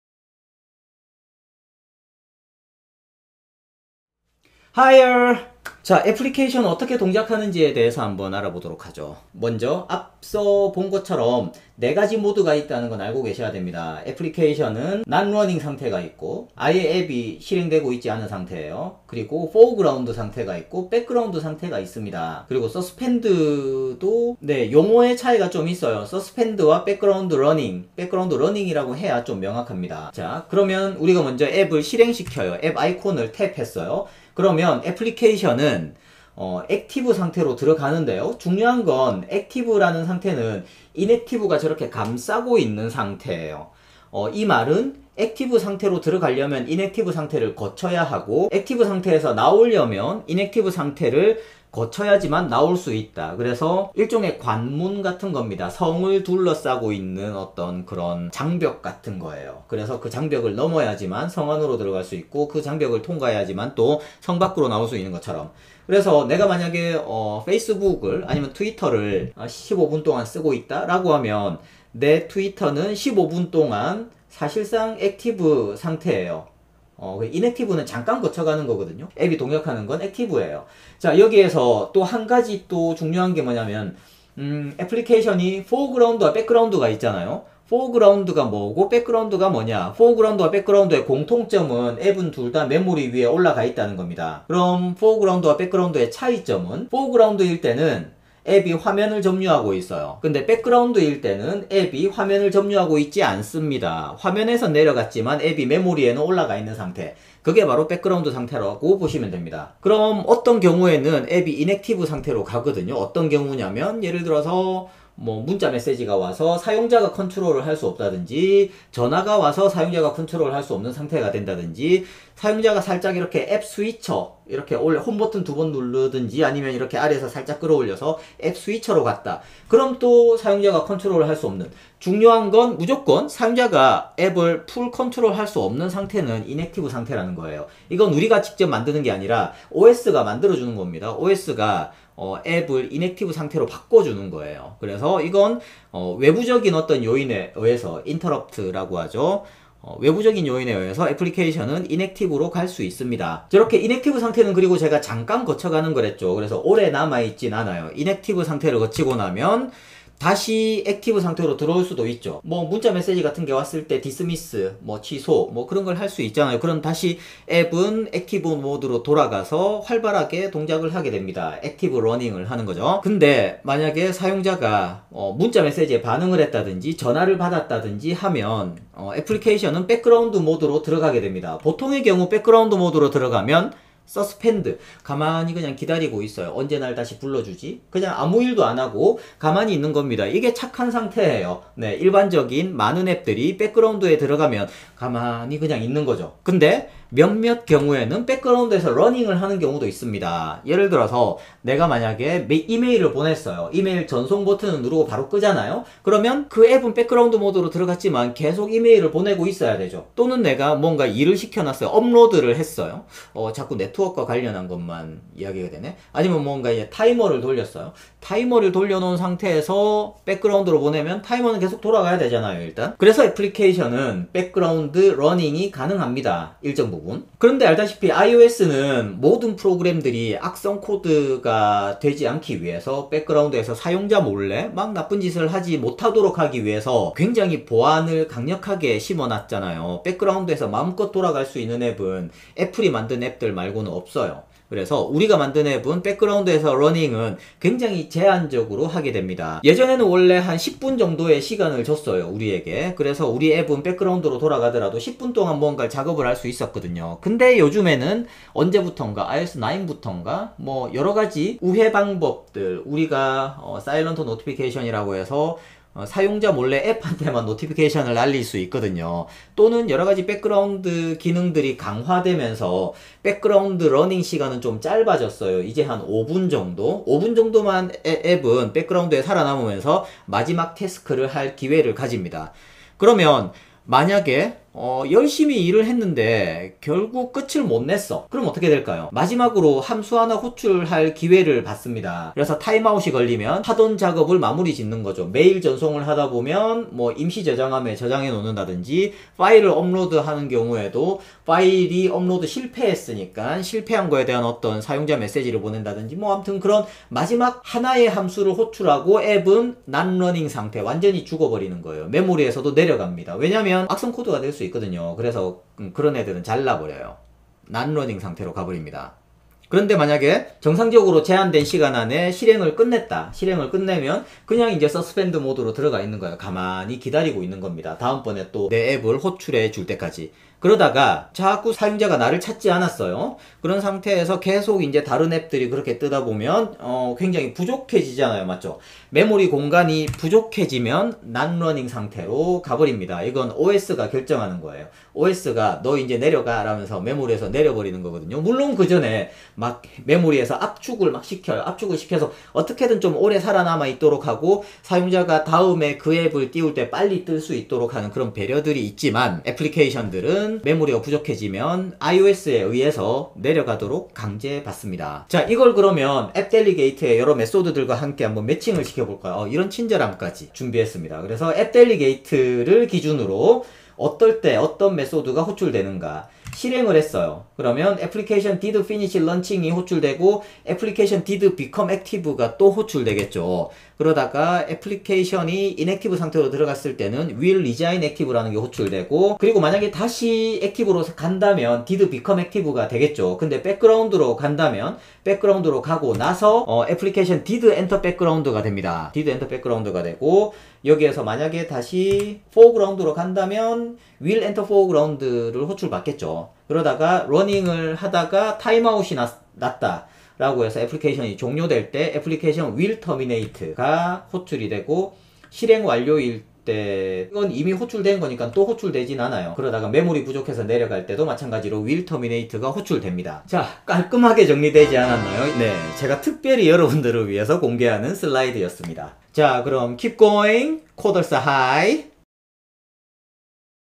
개발자 교양필수 : iOS 앱의 라이프사이클. iOS 앱이 실행 된 후 종료하기까지 거치는 변화. 개발자만이 아니라 기획자 디자이너도 알아야 하는 앱 상식
이 영상은 iOS 앱 개발 기본 ( App Development with Swift ) 동영상에서 발췌한 것입니다.
전체 과정 정보는